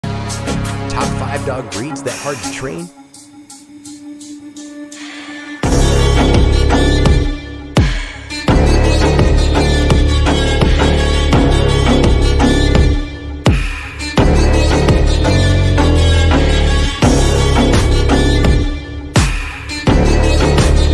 Top 5 dog breed that hard to train
Brace yourselves to discover the top five dog breeds that pose the greatest training challenges. The process of training a dog can be fulfilling, yet it's an open secret that some breeds exhibit more willfulness and independence than others. These breeds necessitate a decisive, patient approach, complemented by a profound comprehension of their distinct temperaments. In this piece, we'll navigate the realm of dog training, delving into the breeds that are notorious for challenging even the most seasoned trainers. Ranging from the spirited Chihuahua to the obstinate Akita, each breed comes with its own unique hurdles and idiosyncrasies. Whether you're pondering over welcoming one of these breeds into your home, or just intrigued about the training dynamics, join us as we reveal the strategies to successfully harness the prowess of these strong-minded pooches.